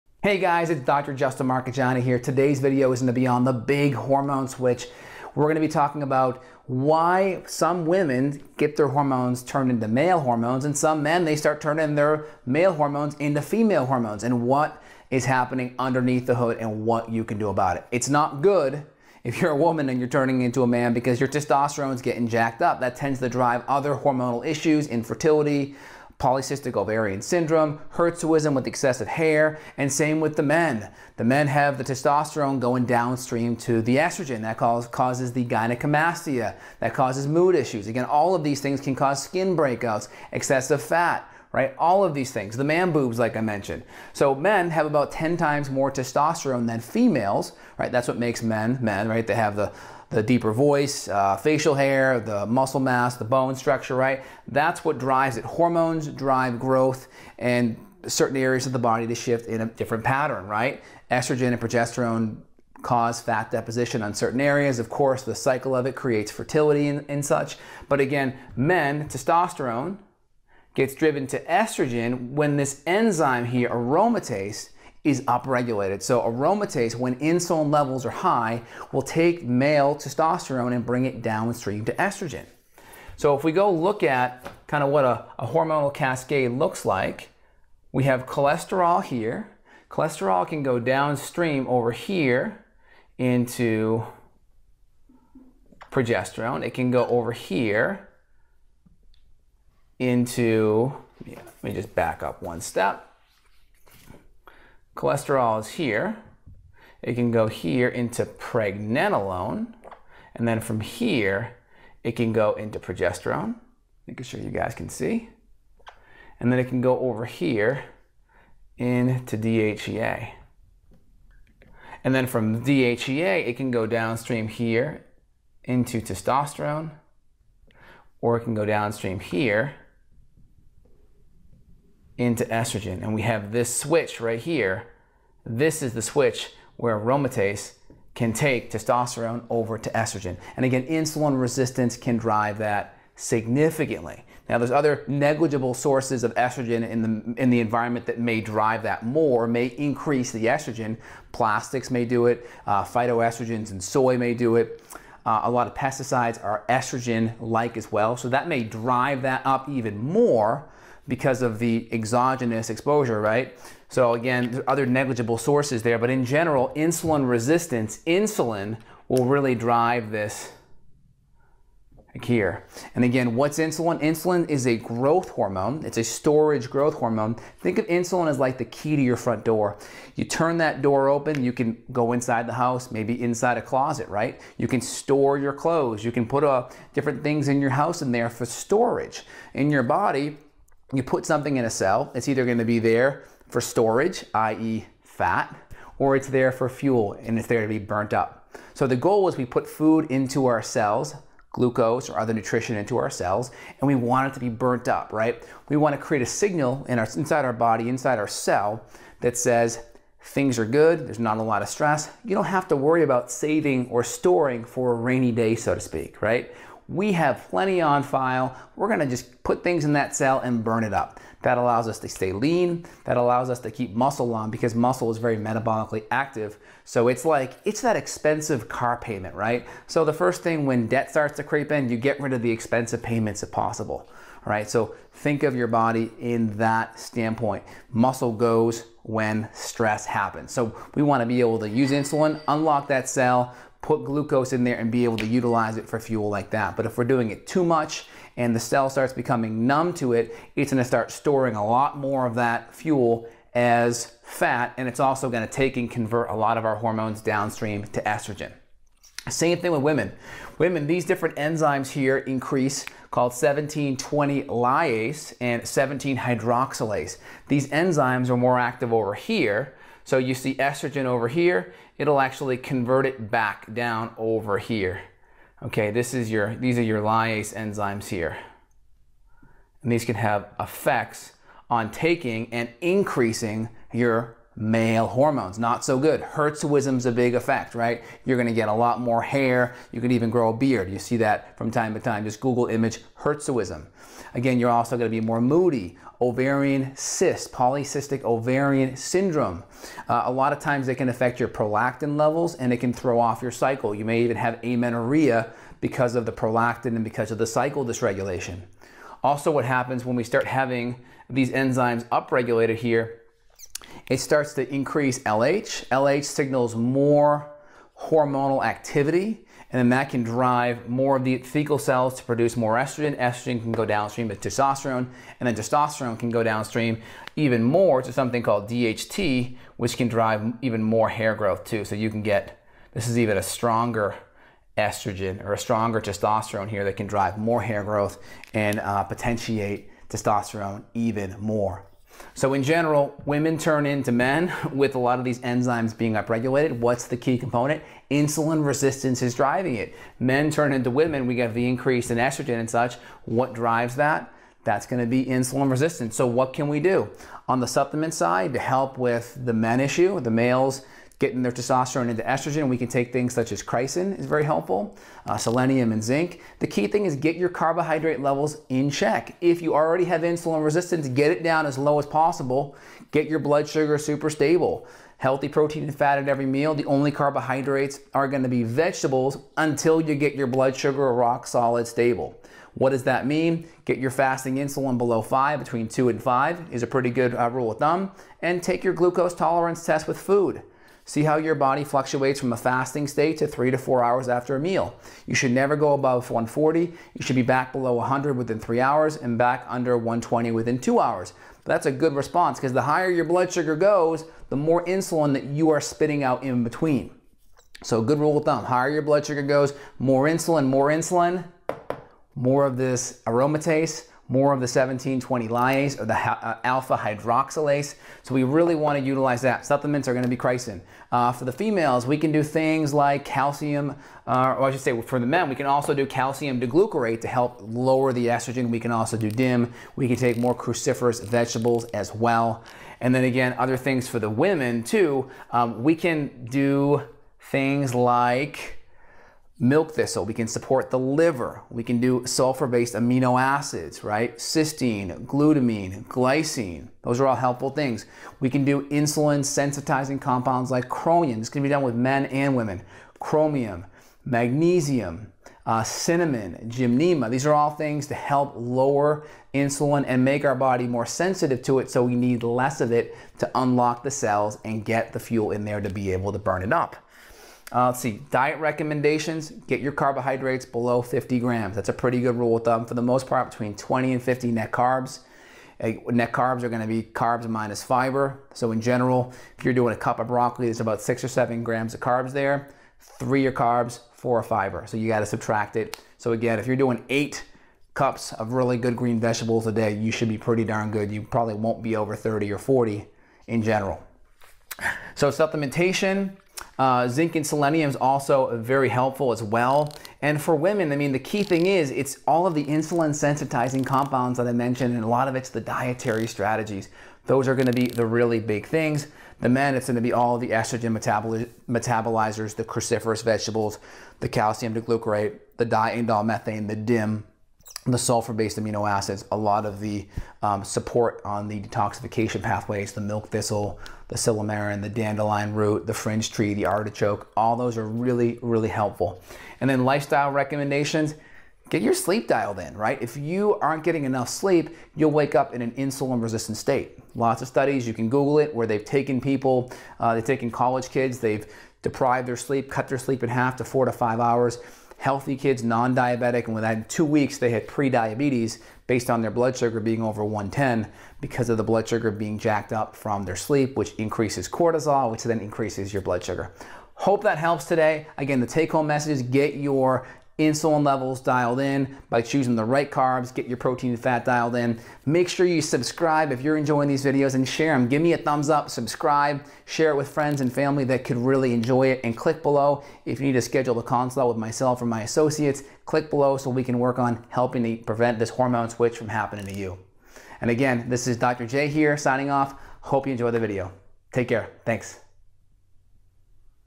Insulin Resistance and the Hormone Switch Aromotase, Estrogen and PCOS
In this video, Dr. Justin Marchegiani talks about the insulin resistance as well as the Aromatase enzyme which causes your hormones to "switch" e.g. it turns the estrogen hormone into testosterone or vice versa. Understand how and why some female hormones change into male hormones and find out how your body responds to this switch. He also answers questions like "what is insulin resistance," how it affects your overall health, how being insulin resistant increases your chance of developing polycystic ovary syndrome (PCOS) and the natural solutions that you can use to prevent or treat insulin resistance.

Check out the video to learn more about this health issue.